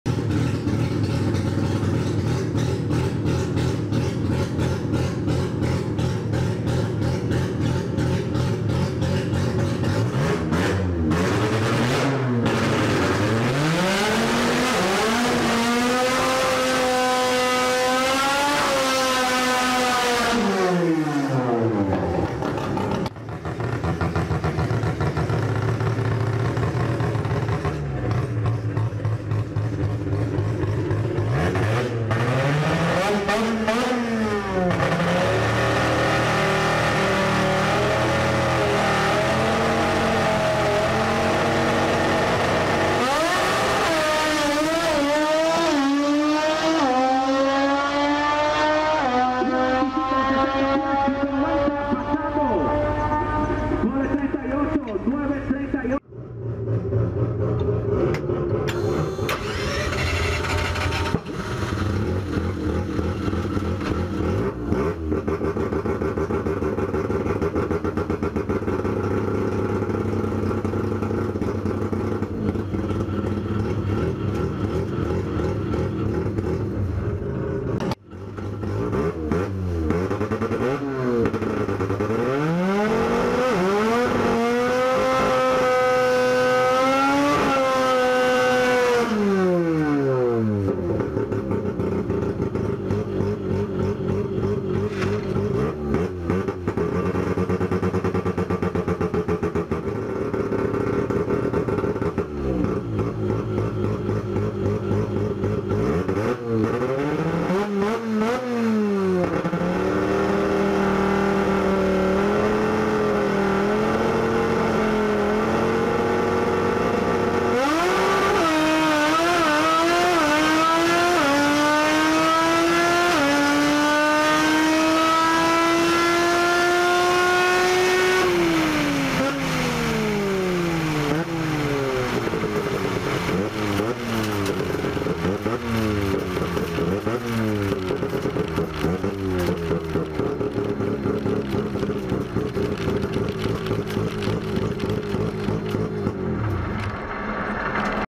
THE NEW JANYi
EVENTO ROTORES VS PISTONES 27 DE MAYO 2017 EN SALINASSPEEDWAY.

Grabado por siloe studio buscanos en facebook